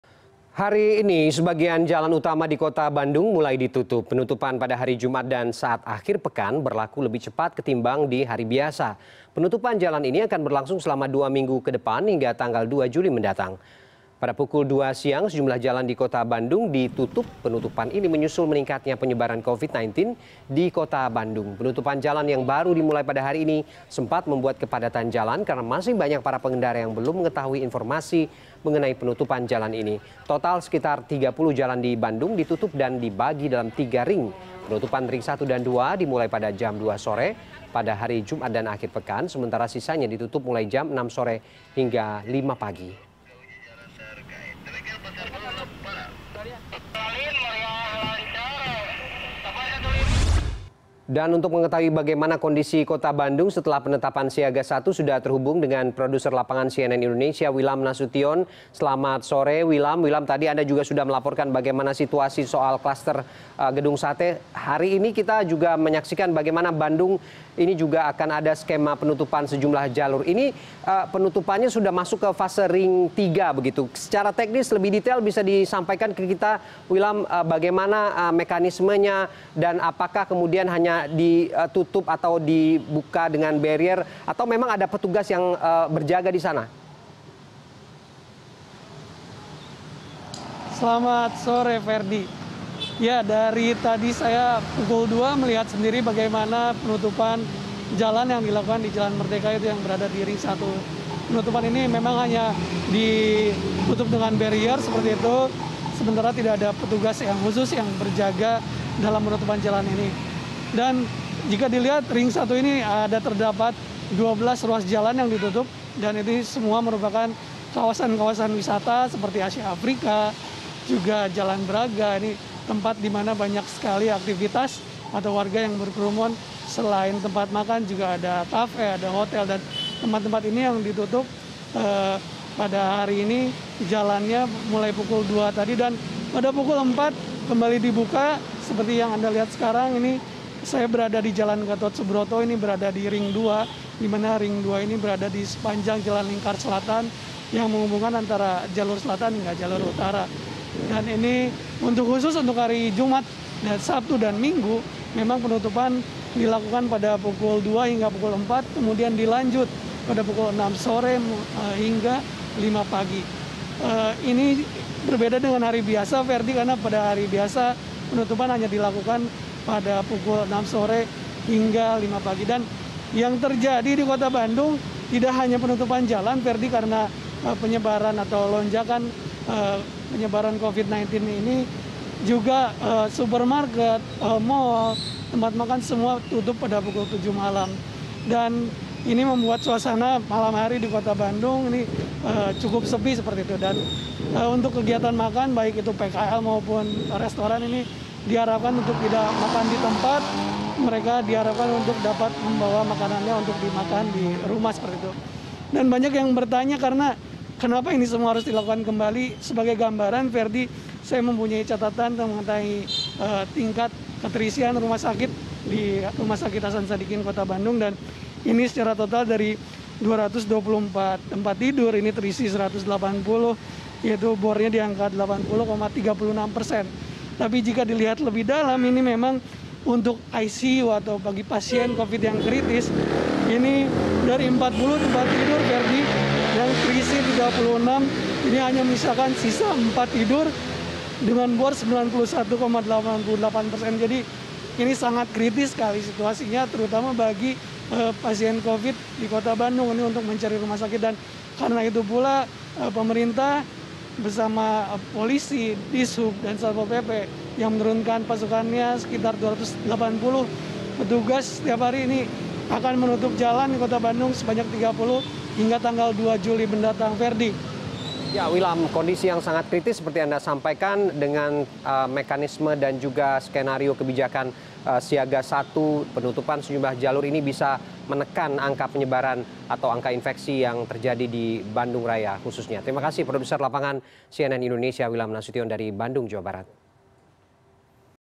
Bandung Siaga Satu, Sejumlah Jalan Ditutup
Hari ini sebagian jalan utama di Kota Bandung mulai ditutup. Penutupan pada hari Jumat dan saat akhir pekan, berlaku lebih cepat ketimbang di hari biasa. Penutupan jalan ini akan berlangsung selama 2 minggu ke depan hingga tanggal 2 Juli mendatang.